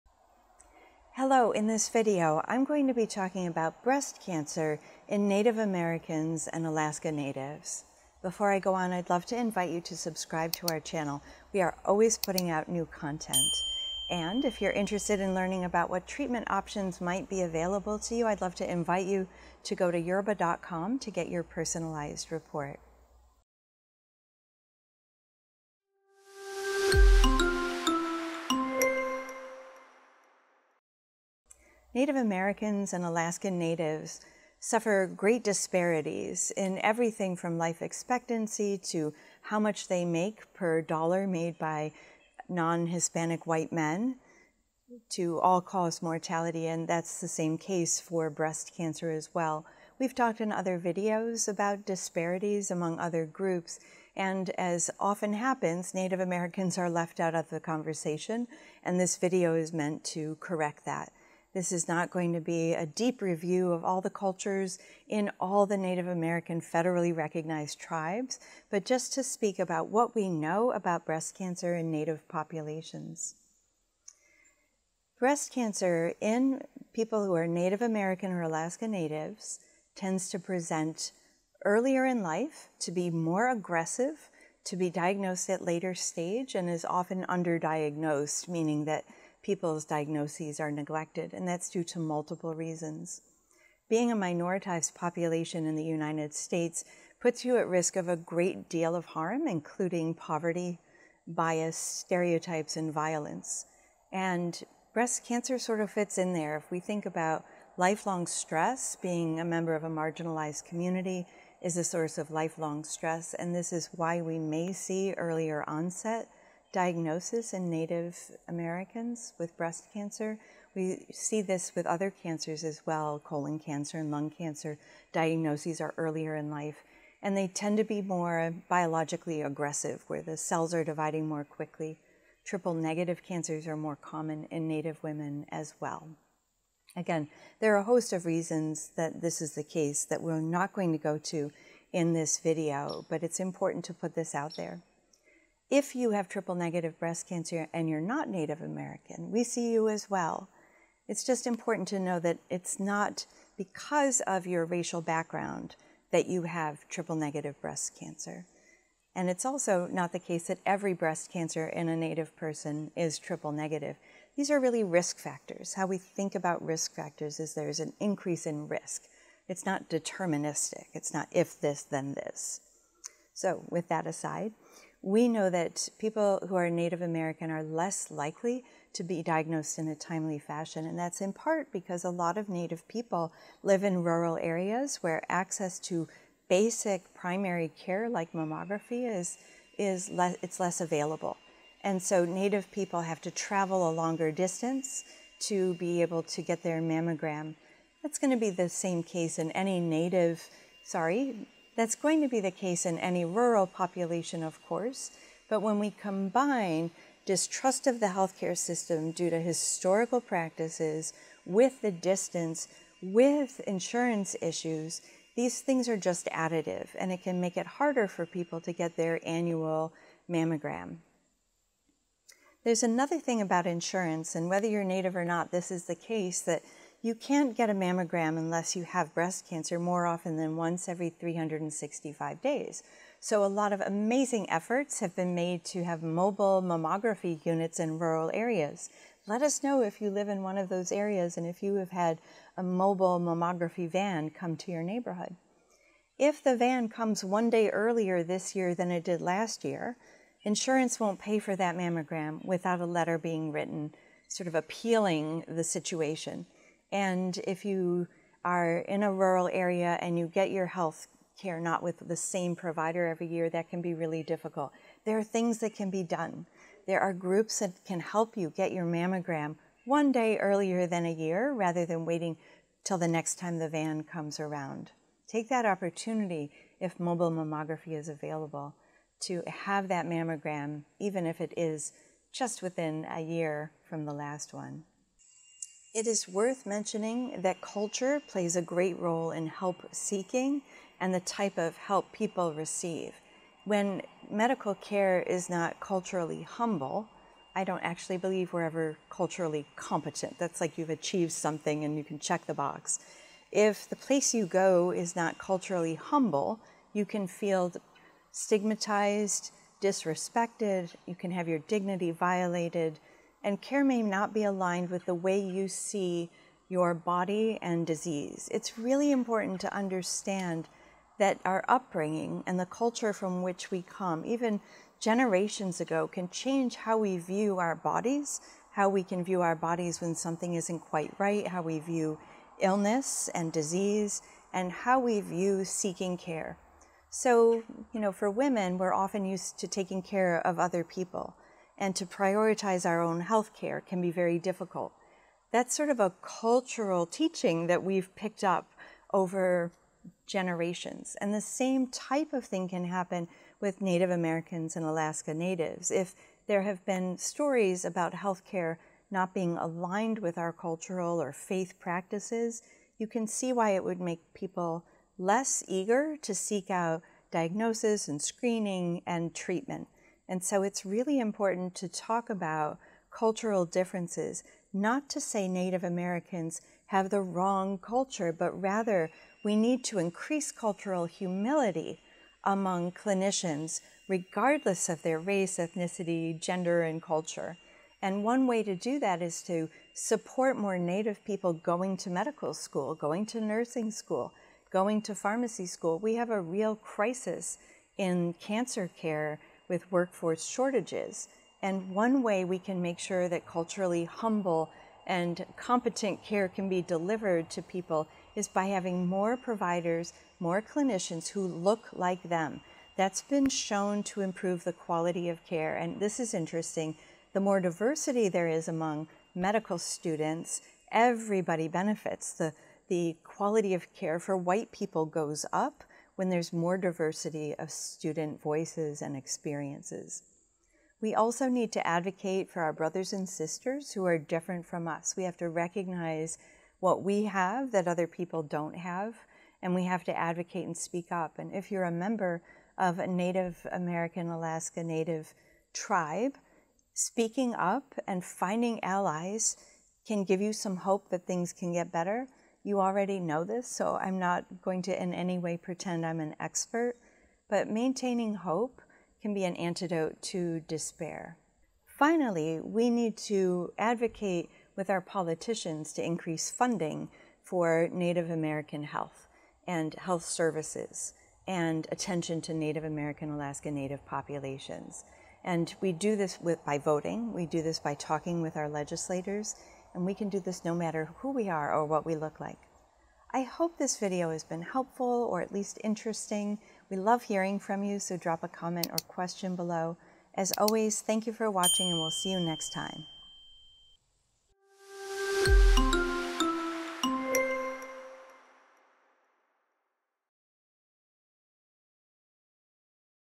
Breast Cancer in Native American and Alaskan Native Women: Disparities in Care
Why do Native American and Alaska Native women face higher breast cancer mortality rates? How do delayed diagnoses, cultural barriers, and systemic inequities play a role? In this video, Dr. Jennifer Griggs discusses why breast cancer outcomes are worse among Native populations, the factors behind later‑stage diagnoses, and how improved access to care can save lives. She also shares how advocacy, representation in healthcare, and mobile screening programs can help bridge these gaps.